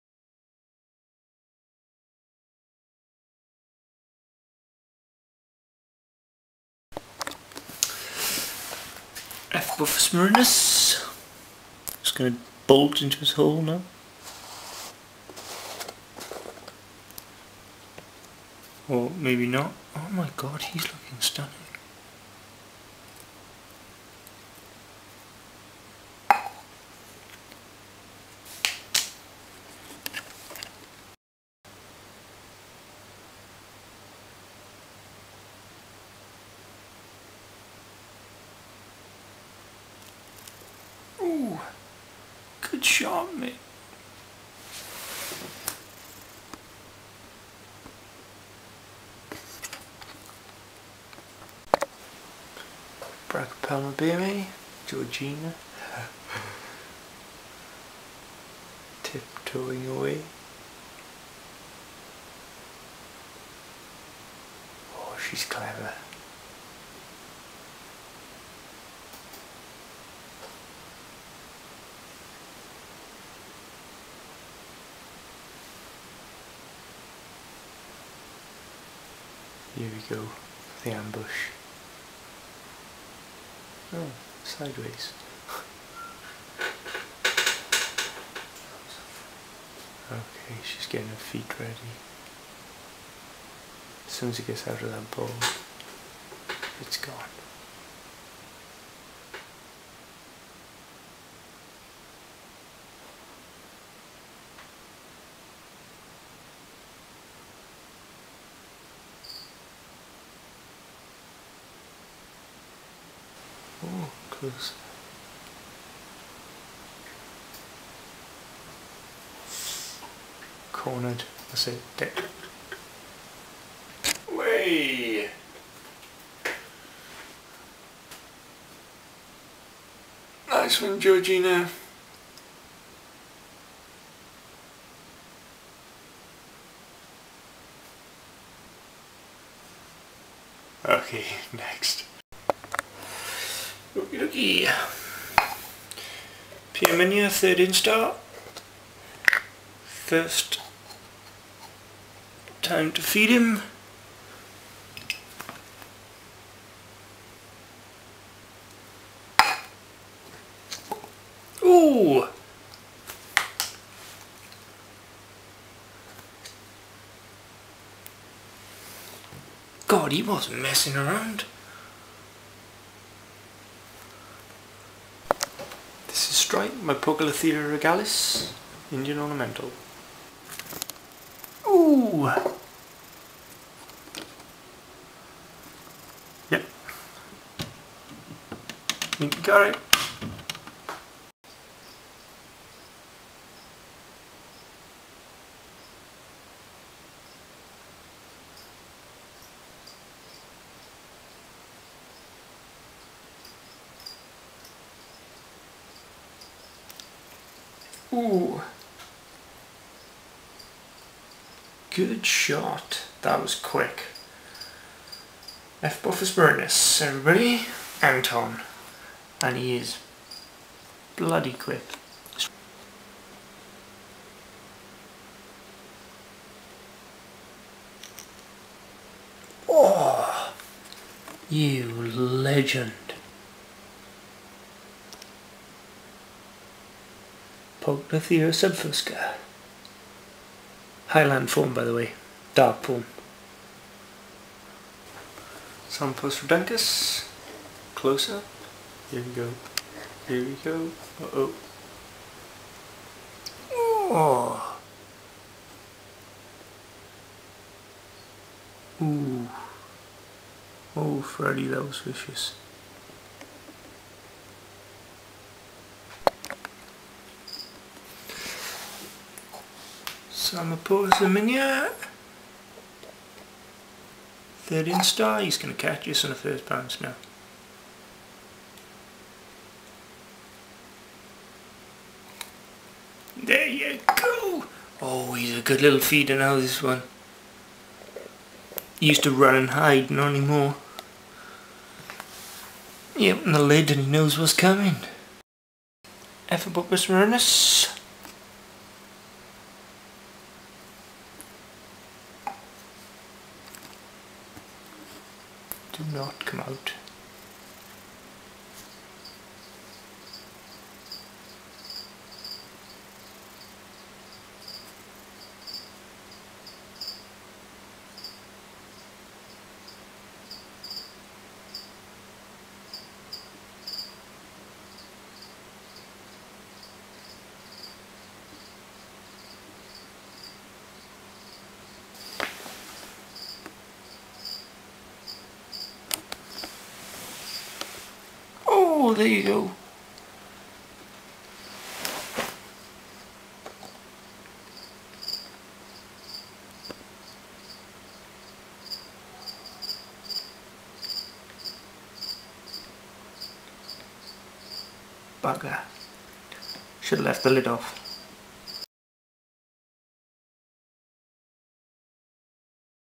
Tarantula feeding video no.7
The 7th feeding video of me feeding some of my tarantulas
Starring:
Ephebopus murinus - Anton
Brachypelma boehmei - Georgina
Psalmopoeus irminia - Oscar
Poecilotheria regalis - Stripe
Psalmopoeus irminia - Lecter
Poecilotheria subfusca - Aleister
Psalmopoeus reduncus - Freddie